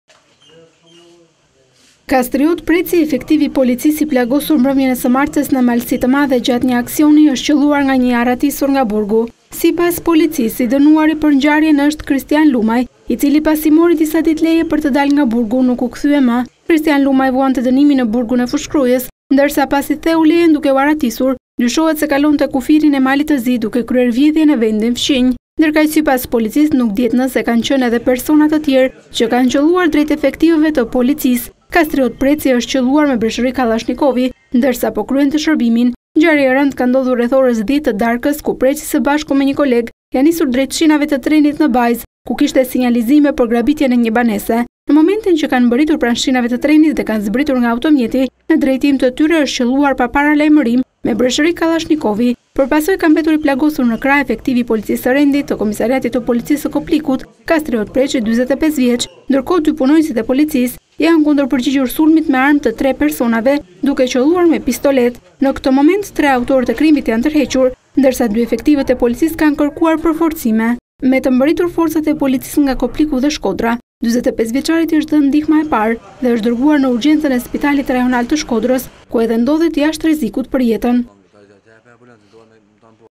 A1 Report - Vidhte në kufi me Malin e Zi, i arratisuri nga burgu qëllon policin
SHKODER- Një efektiv policie është plagosur me armë zjarri mbrëmjen e djeshme në Malësi të Madhe, pasi disa persona i zunë. Ngjarja ka ndodhur rreth orës 22:30 në fshatin Hot, ndërsa dy efektivë policie pasi u njoftuar për një vjedhje shkuan te "Shinat e trenit". Sipas policisë, i dyshuar për ngjarjen është Kristian Lumaj, i cili pasi mori disa ditë leje për të dalë nga burgu, nuk u kthye më.

Krisitan Lumaj vuante dënimin në burgun e Fushë-Krujës, ndërsa pasi theu lejen duke u arratisur, dyshohet se Lumaj kalonte kufirin e Malit të Zi, duke kryer vjedhje në vendin fqinj.

Ndërkaq, sipas policisë nuk dihet nëse kanë qenë edhe persona të tjerë që kanë qëlluar drejt efektivëve të policisë.

I zunë pritë, plagoset polici
Kastriot Preci është qëlluar me breshëri kallashnikovi ndërsa po kryente shërbimin. Ngjarja e rëndë ka ndodhur rreth orës 22:30 kur 3 persona akoma të paidentifikuar kanë marrë në telefon sallën operative të komisariatit të Malësisë së Madhe dhe kanë njoftuar se pranë shinave të trenit në Bajzë po grabitet një banesë.

 Ngjarja ka ndodhur rreth orës 22:30 në fshatin Hot, ndërsa dy efektivë policie pasi u njoftuar për një vjedhje shkuan te "Shinat e trenit".

Drejt vendit të ngjarjes janë nisur menjëherë grupi i gatshëm i dy punonjësve të policisë, mes të cilëve ka qenë edhe efektivi i policisë Kastriot Preci. Në momentin që kanë mbërritur pranë shinave të trenit dhe kanë zbritur nga automjeti në drejtim të tyre është qëlluar pa paralajmërim me breshëri kallashnikovi.

Për pasojë ka mbetur i plagosur në krah efektivi i policisë së rendit të komisariatit të policisë së Koplikut Kastriot Preci 45 vjeç. Ndërkohë 2 punonjësit e policisë i janë kundërpërgjigjur sulmit me armë të 3 personave duke qëlluar me pistoletë. Në këtë moment 3 autorët e krimit janë tërhequr ndërsa 2 efektivët e policisë kanë kërkuar përforcime. Me të mbërritur forcat e policisë nga Kopliku dhe Shkodra, 45-vjeçarit i është dhënë ndihma e parë dhe është dërguar në urgjencën e spitalit rajonal të Shkodër të s ku edhe ndodhet jashtë rrezikut për jetën.

Sipas bluzave të bardha 45-vjeçari është qëlluar në krah, ndërsa predha e plumbit i ka shkaktuar edhe frakturë. Nga ana tjetër në vendngjarje kanë mbërritur forca të shumta policie të cilat janë vënë në ndjekje të autorëve.

Grupi hetimor po punon intensivisht për zbardhjen e plotë të shkaqeve të kësaj ngjarjeje të rëndë.